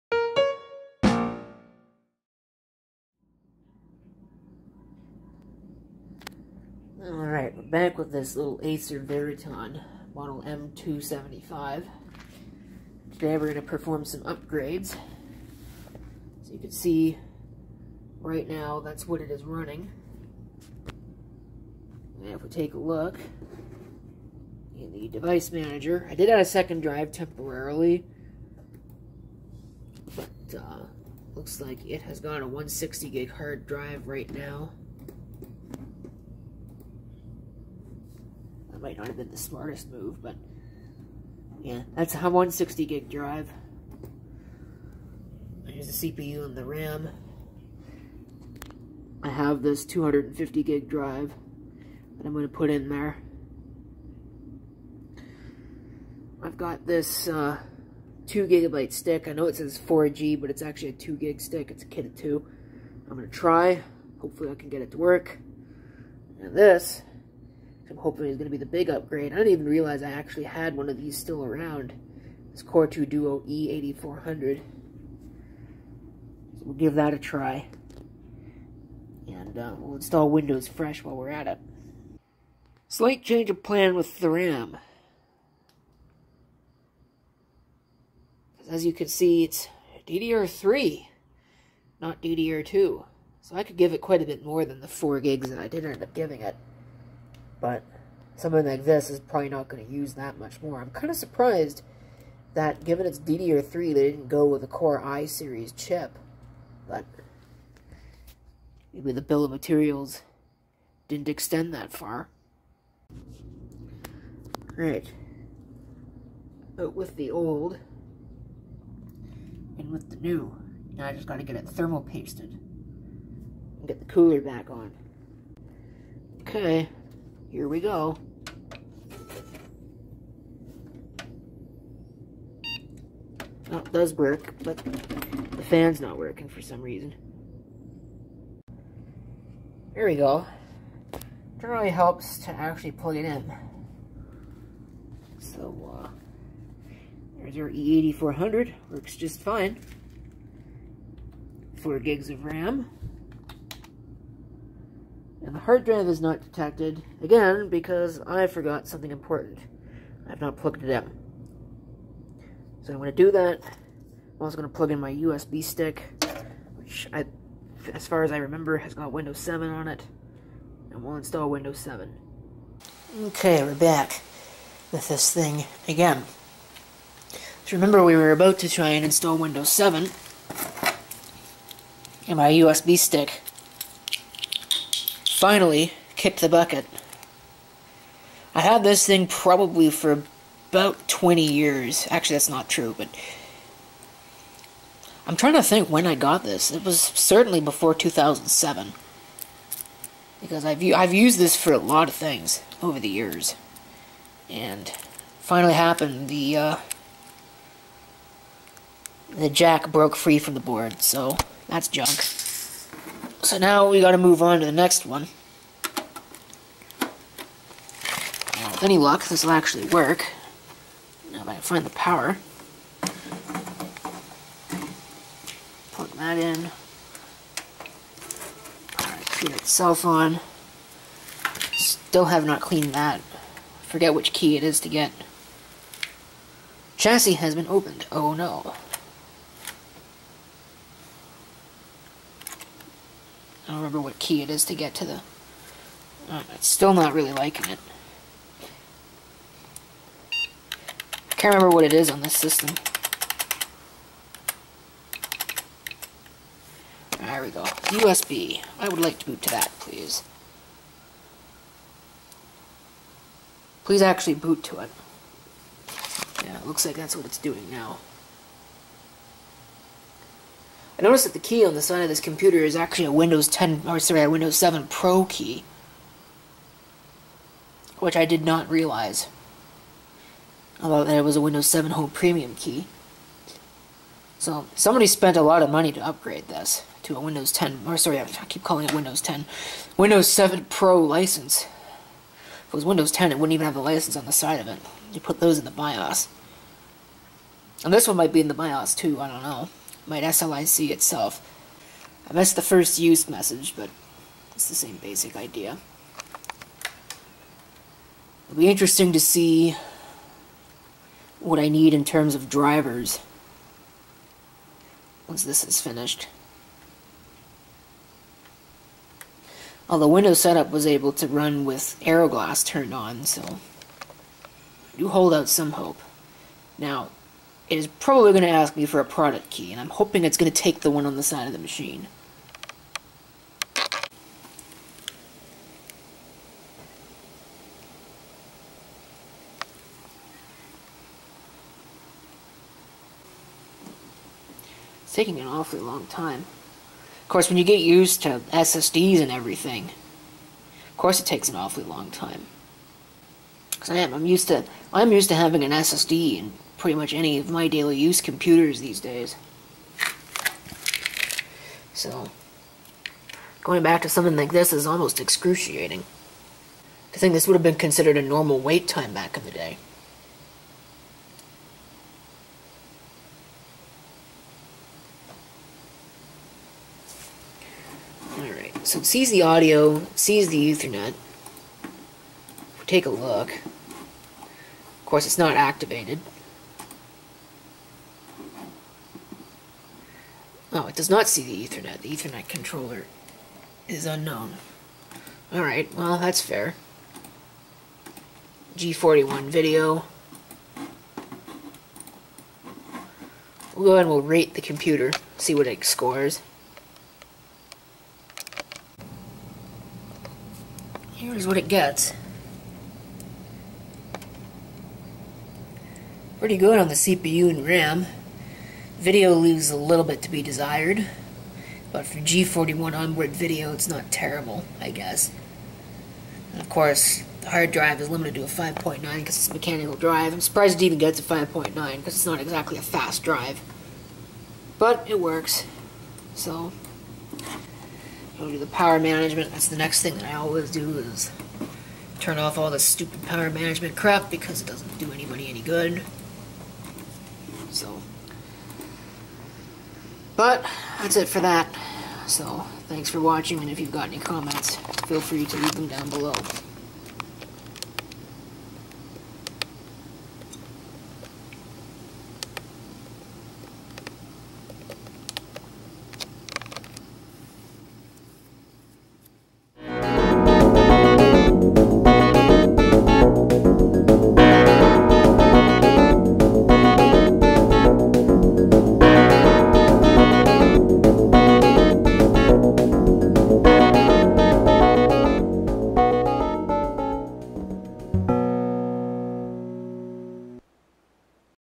Acer Veriton M275 Upgrades
CPU, RAM and hard drive upgrades, plus a fresh Windows 7 install.